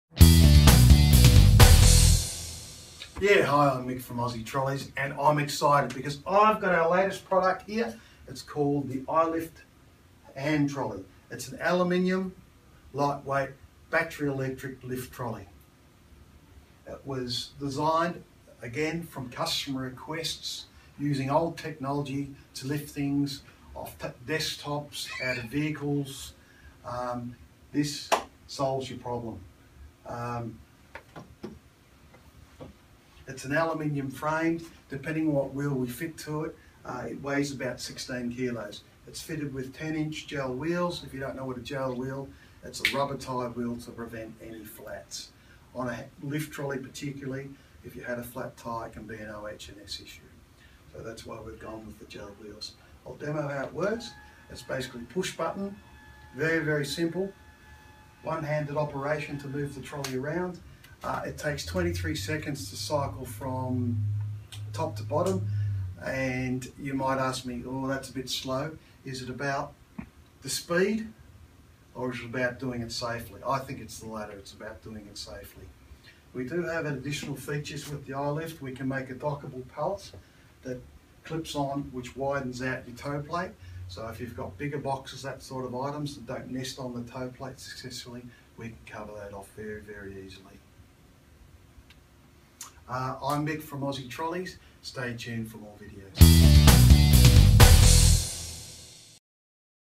The Aussie Trolleys iLift Electric Hand Trolley
One of our most technologically sophisticated trolleys.
Our iLift Electric Hand Trolley is a masterpiece of engineering and capability.

Lifts heavy loads quickly and safely.

Model: AT/LIFT
Load: Static 150kgs Lift 80kgs
Dimensions: W 340 mm   H 1180 mm
Description:
An Aluminium Framed, Lightweight Battery Powered Lifting Device. Australian designed & made. Patent Pending.
Ideal for the office, factory or warehouse.
Great for loading from vans, utes & trucks.
Lift height:- 8mm to 740mm. Toe plate:- 350mm X 240mm.
Weight:- 13.5kgs. Wheels:- 2 x 10" / Gel Filled Tyres.
Lifetime warranty on the frame.
Battery electric with built in charger.
Automatic electric overload protection system ensures safety.
Quiet smooth operation & easy push button height adjustment.
Conforms with Electromagnetic Compatible Standards as required under the Radiocommunications Act.
Perfect for transporting & positioning loads quickly & easily.
Highly manoeuvrable, lightweight & portable.Unique forklift style toe plate for use with the mini pallet.

OPTIONS :- Solid & larger toe plate. Ratchet strap. Solid wheels.

For more information on this trolley, please visit our online store...

For more industry leading materials handling solutions, check out our web site at...